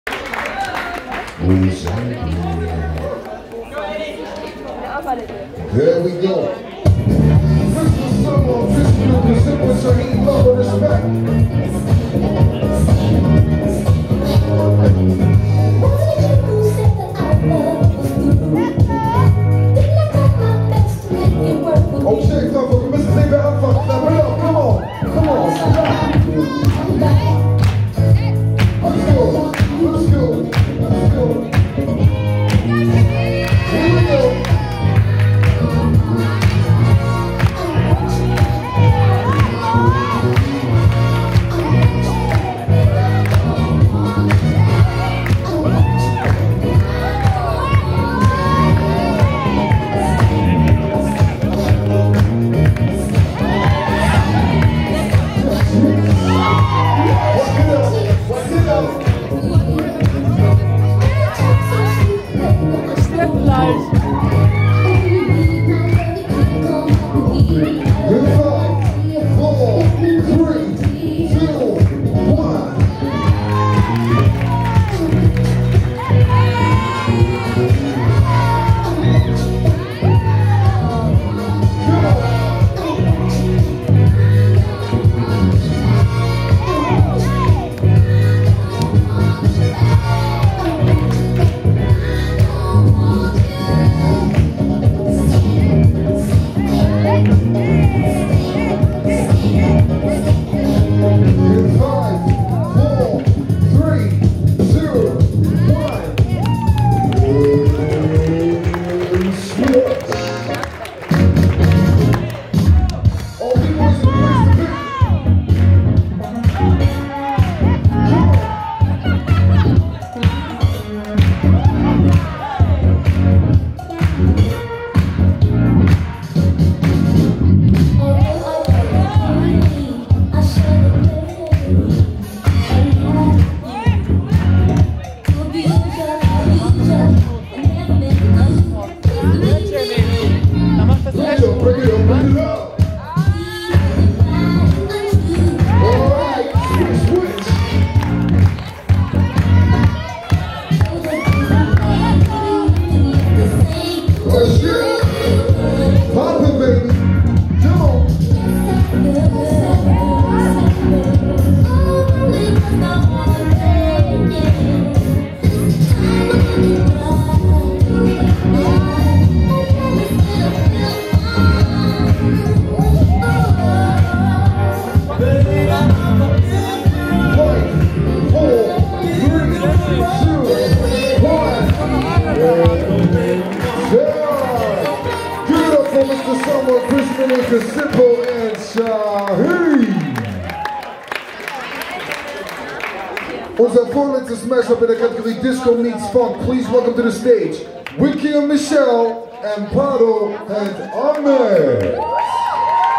Urban Episode 2019 x Disco meets Funk Preselection x Shahin & Zippo vs. Krissy & Mr. Someone
Urban Episode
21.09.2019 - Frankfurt

Dancers:
Shahin
Zippo (DopeRoc)

Krissy
Mr. Someone (Groove68)

DJ:
DJ Crash